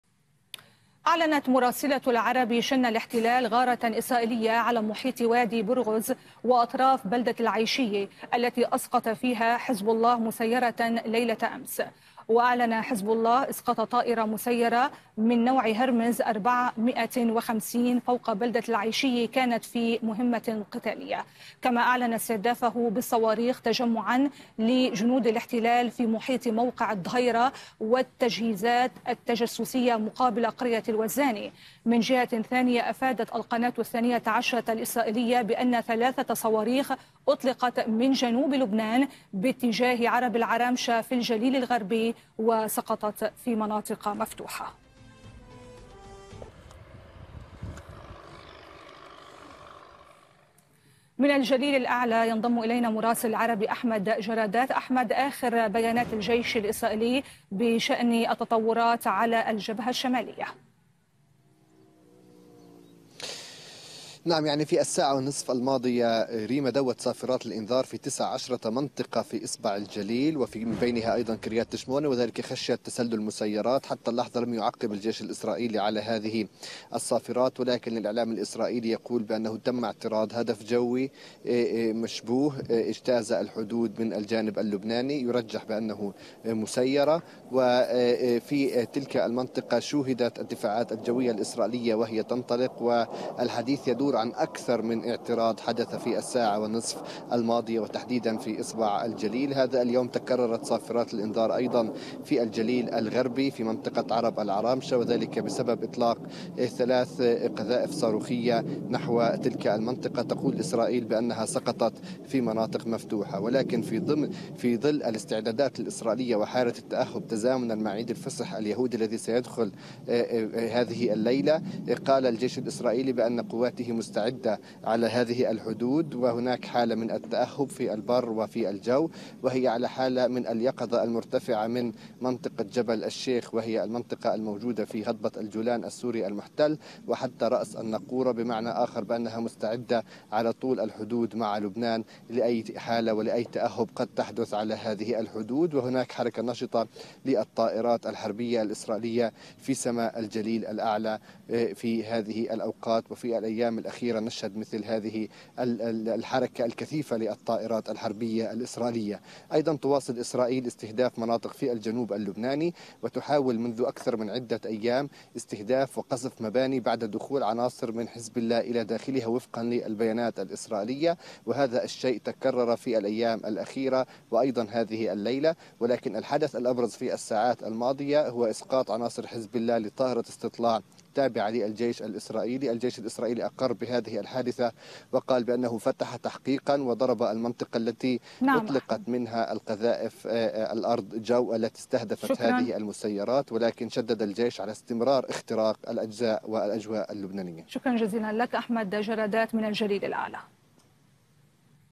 حزب الله يستهدف تجمعا لجنود الاحتلال بالصواريخ وصفارات الإنذار تدوي في 19 منطقة شمال إسرائيل
أفاد مراسل التلفزيون العربي في الجليل الغربي أحمد جرادات بأن صفارات الإنذار دوت في 19 منطقة في شمال إسرائيل للاشتباه في تسلل مسيّرات، مشيرا إلى أن الإعلام الإسرائيلي أكد اعتراض جسم مشبوه.